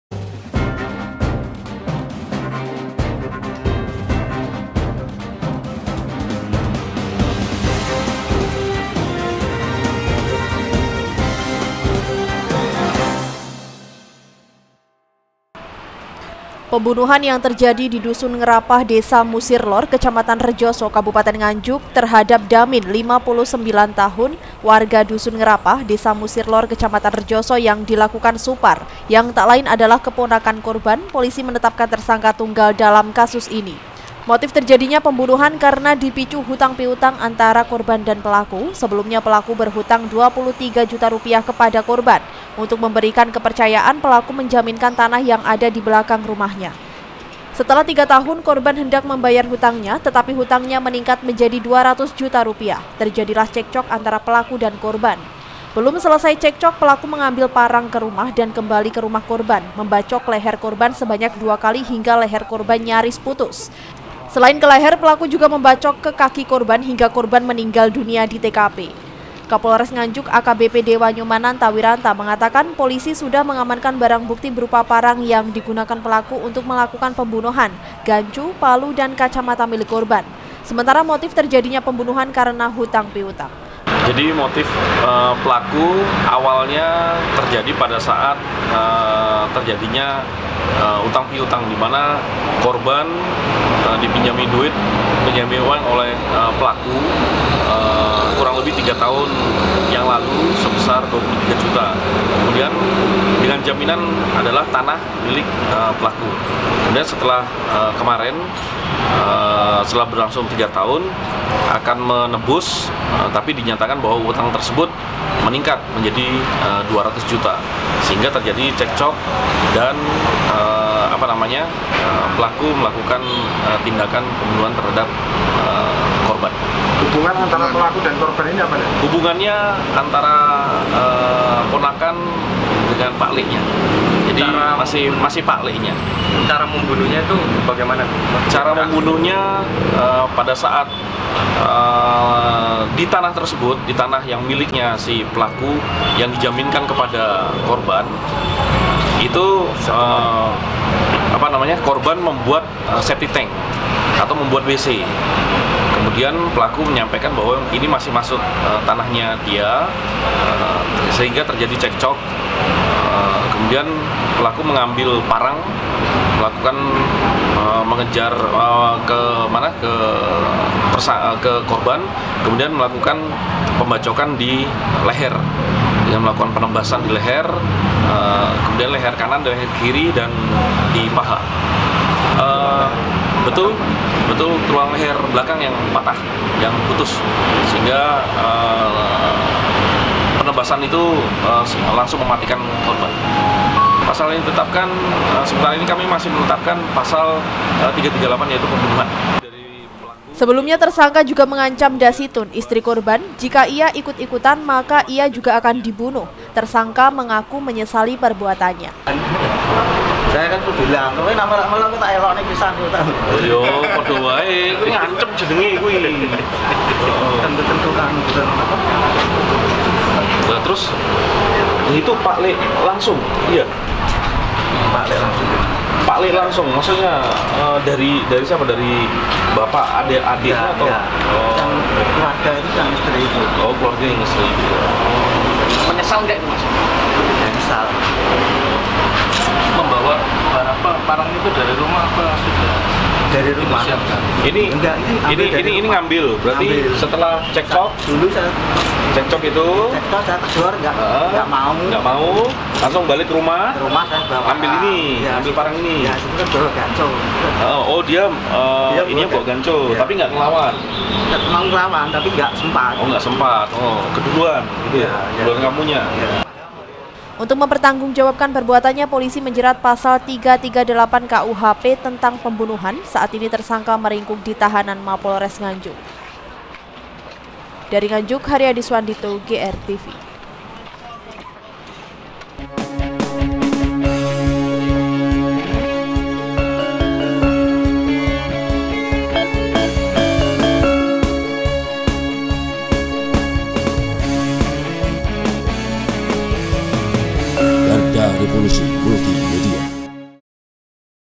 NGANJUK - Polisi Tetapkan Tersangka Tunggal Kasus Keponakan Bunuh Paman
pesan dan beli kaos garda warta spesial edition berbagai ukuran dan nyaman dipakai hubungi  : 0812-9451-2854 (afif ari) atau kunjungi